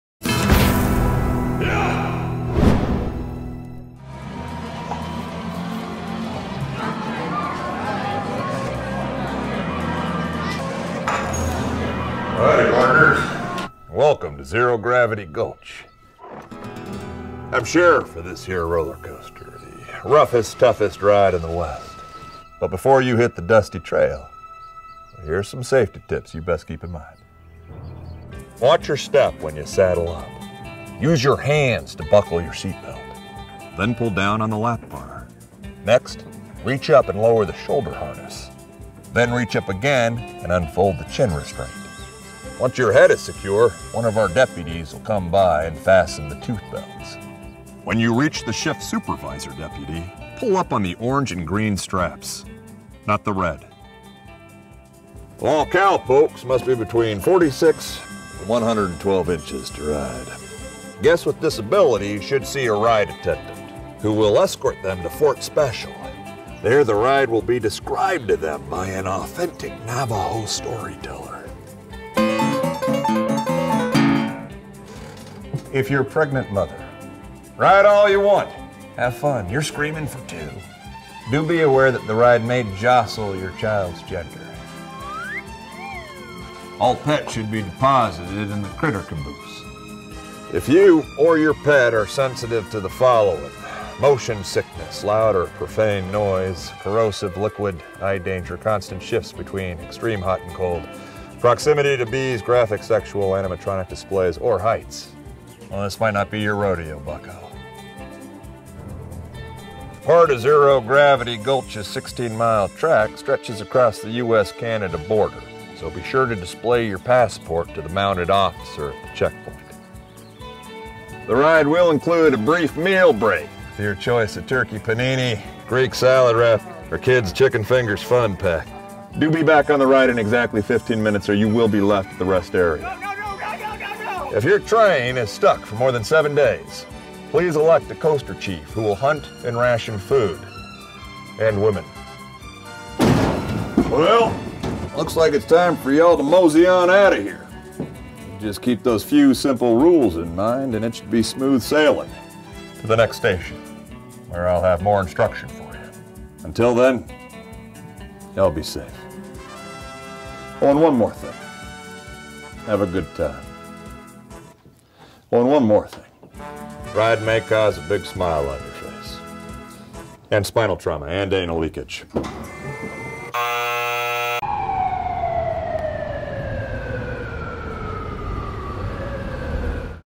Rollercoaster Safety with Patrick Warburton
Before you ride this 100% not dangerous roller coaster, sheriff Patrick Warburton has a few simple instructions for ya.